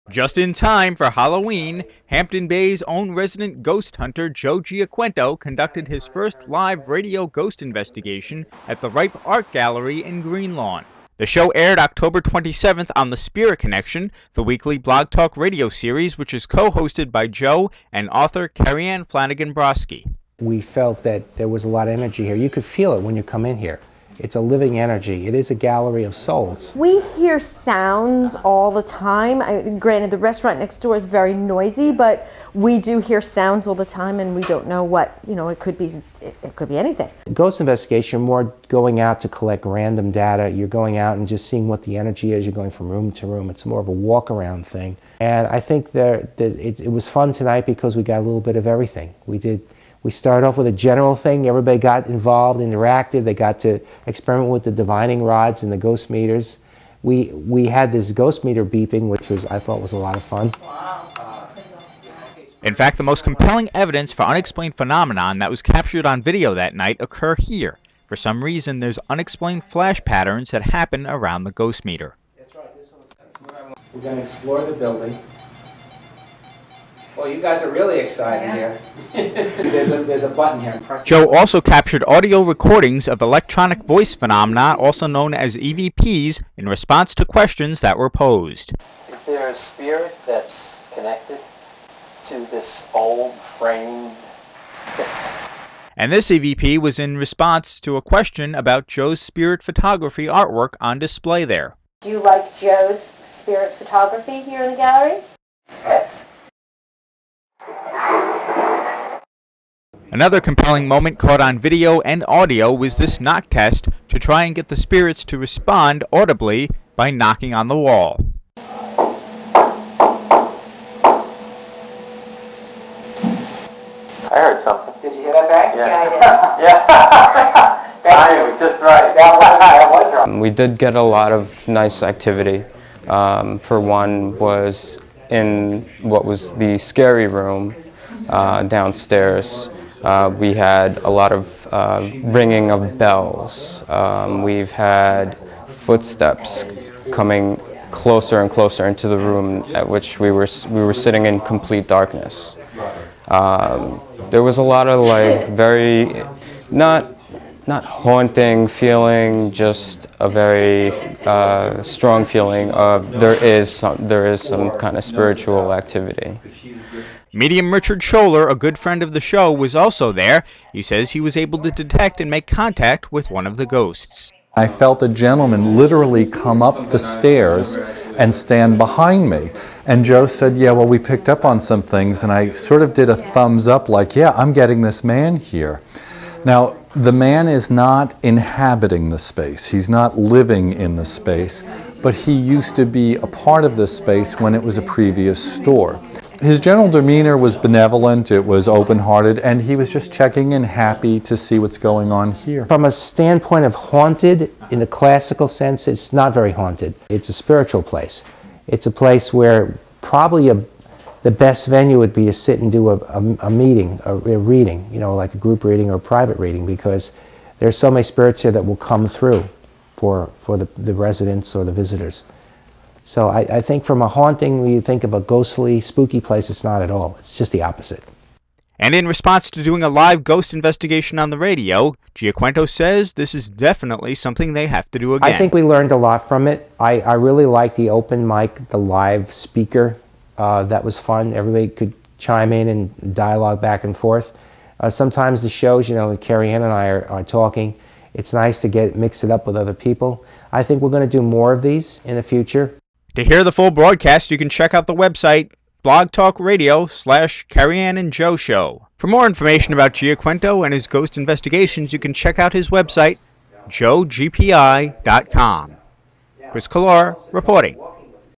Ghost Investigation in Greenlawn
Chris Collora reports on a Halloween Ghost Investigation at an suspected Haunted Art Gallery in Greenlawn